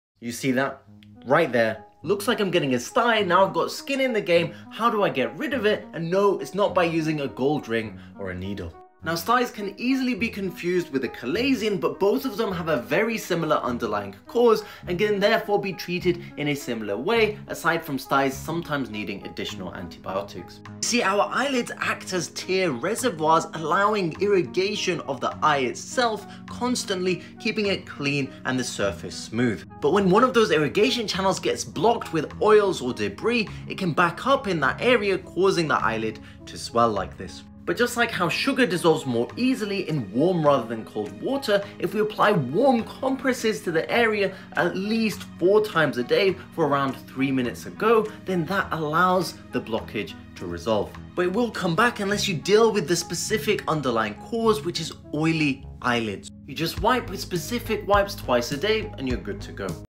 “My Eyelids Keep Getting Bumps”
If oily eyelids are not properly managed, they can create a cycle of persistent swelling and irritation. The excessive oil produced by the meibomian glands in the eyelids can trap debris, bacteria, and dead skin cells along the lash line. This buildup can clog the glands and lead to inflammation, a condition known as meibomian gland dysfunction (MGD). Over time, this may cause chronic eyelid swelling, redness, and discomfort.

The oily residue can also exacerbate conditions such as blepharitis, an inflammation of the eyelid margins, which further irritates the delicate tissues. Left untreated, this environment fosters bacterial growth and even secondary infections. The swelling may persist or worsen as the glands struggle to function properly, and the eyelid tissues remain inflamed.

Additionally, excessive oiliness can interfere with the stability of the tear film, leading to dry eye syndrome, which can make the eyes feel gritty and exacerbate swelling. Addressing the root cause, such as maintaining proper eyelid hygiene, using warm compresses, or seeking professional treatment for MGD or blepharitis, is essential to break this cycle and restore healthy eyelid function.